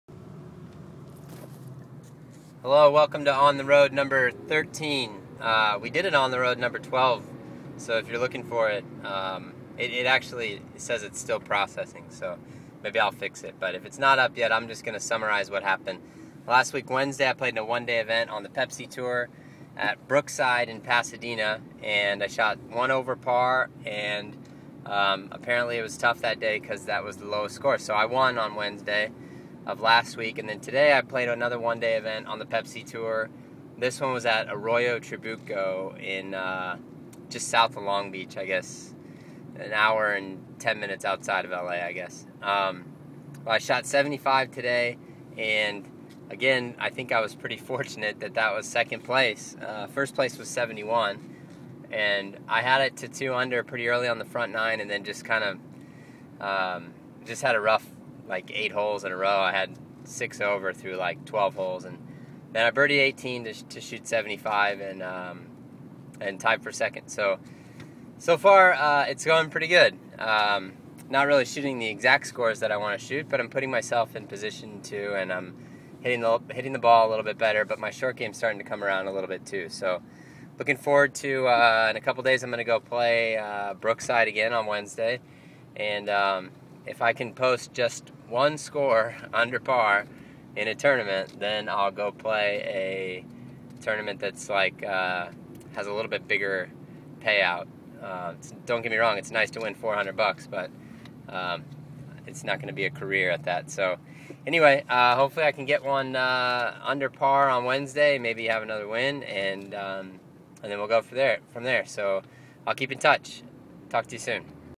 Sam Goulden Golf - On The Road #13
Monday June 20th. Arroyo Trabuco golf club. 2nd place.